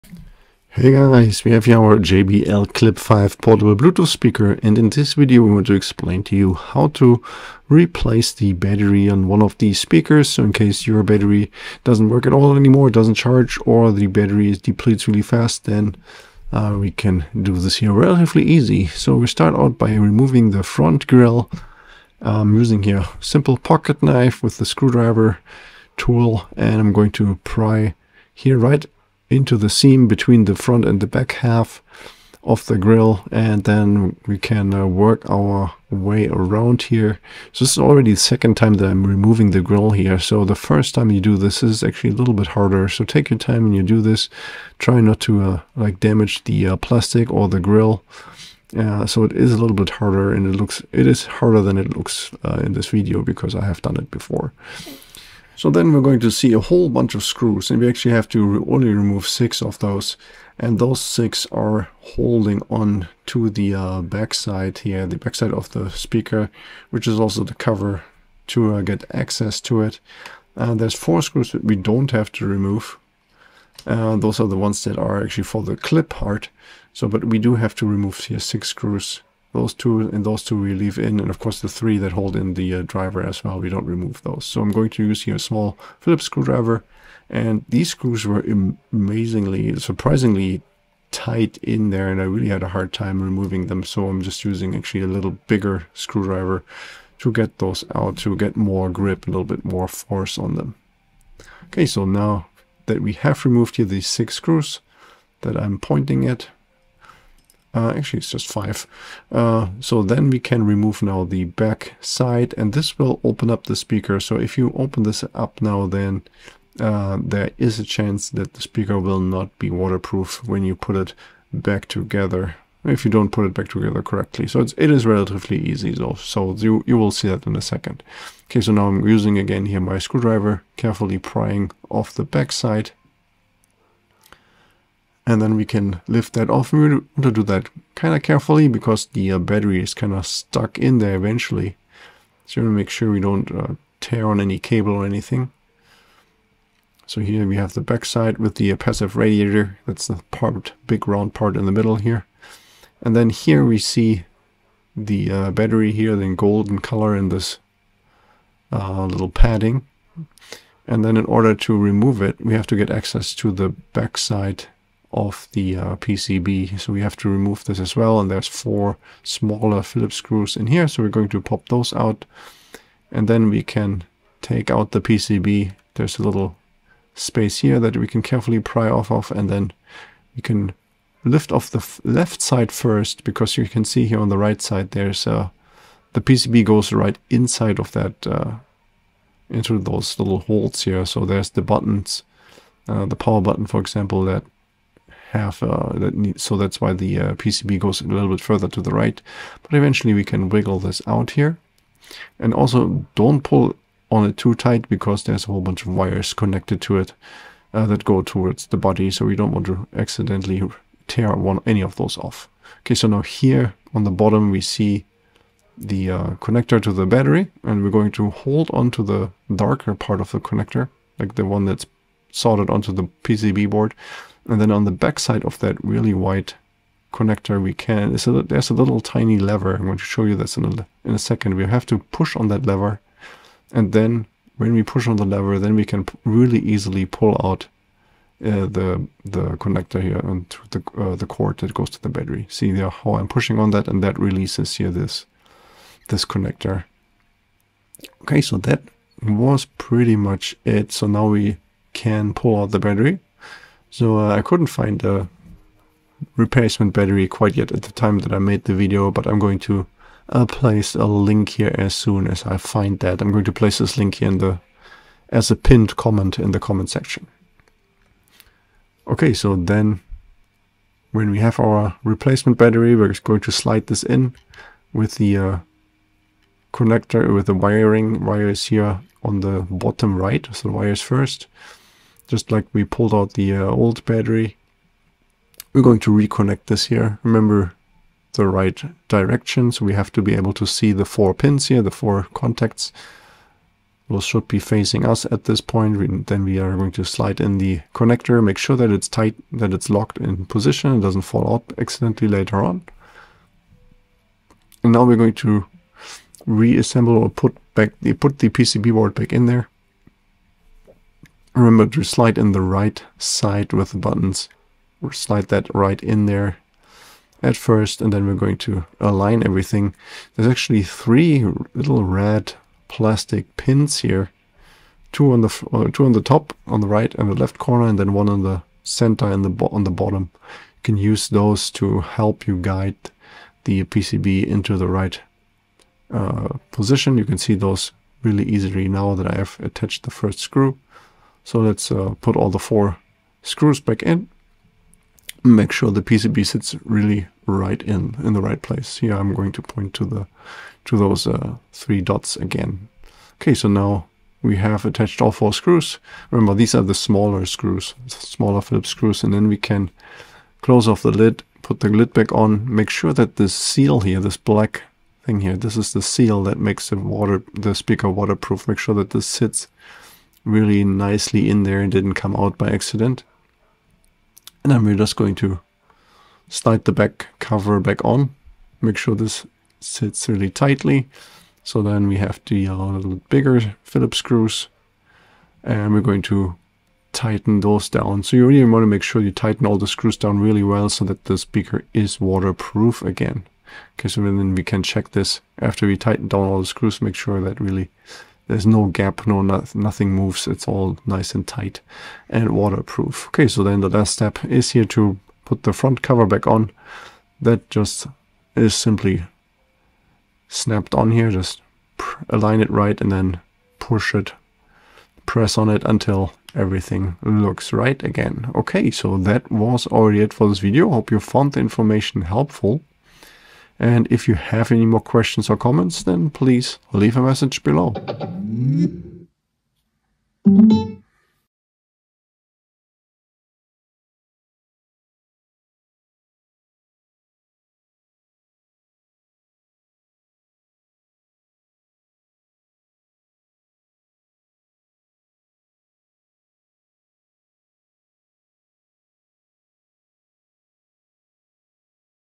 Battery replacement JBL Clip 5 Bluetooth speaker (DIY How to instructions)
In this video we explain in easy to follow, step-by-step instructions how to replace the battery of the JBL Clip5 portable Bluetooth speaker. The process about 15 minutes to complete and requires a small Philips screwdriver. It is fairly easy to perform this repair by yourself.
 We also explain how to put the speaker back together once it is fully disassembled and the battery was replaced.